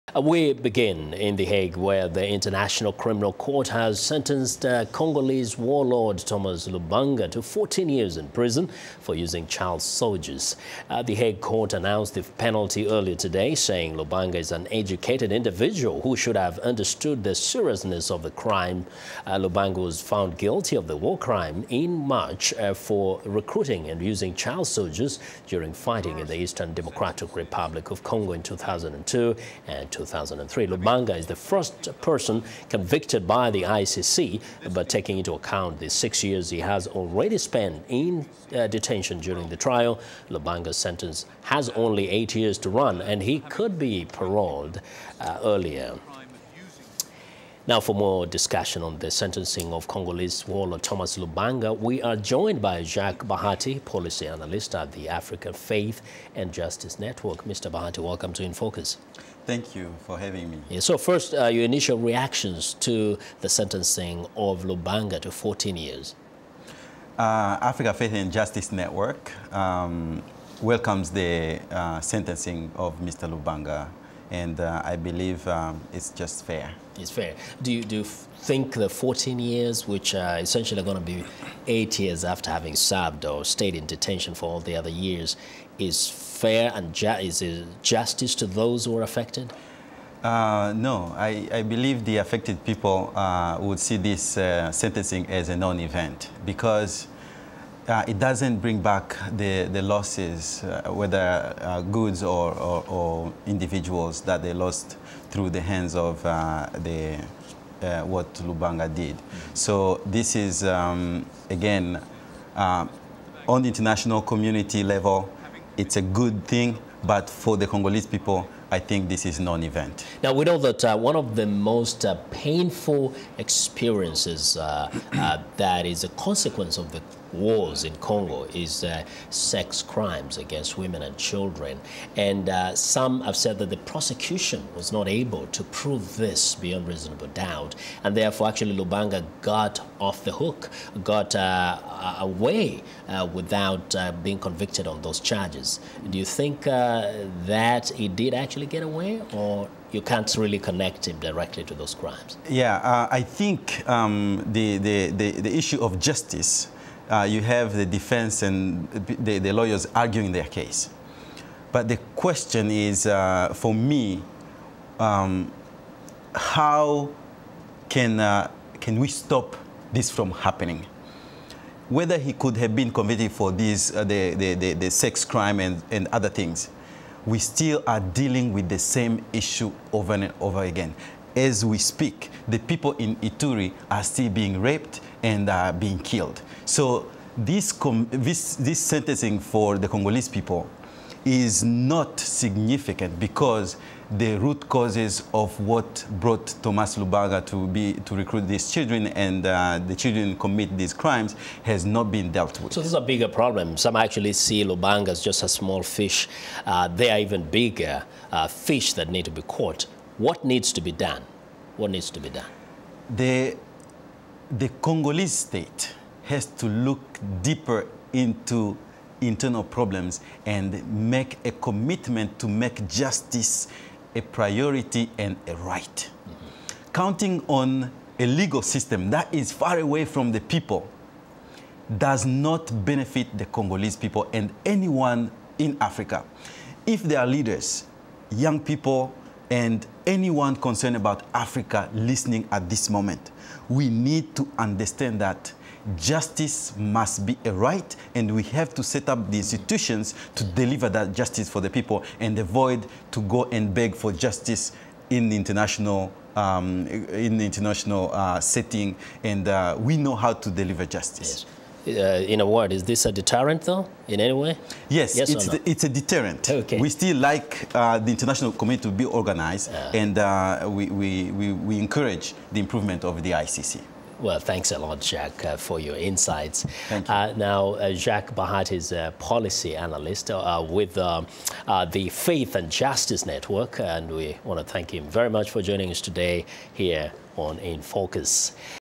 ICC Sentence for Congolese Warlord
The International Criminal Court has sentenced Congolese warlord Thomas Lubanga to 14 years in prison for using child soldiers.  For more discussion on the sentencing, VOA's Vincent Makori talks to Jacques Bahati, policy analyst at the Africa Faith and Justice Network.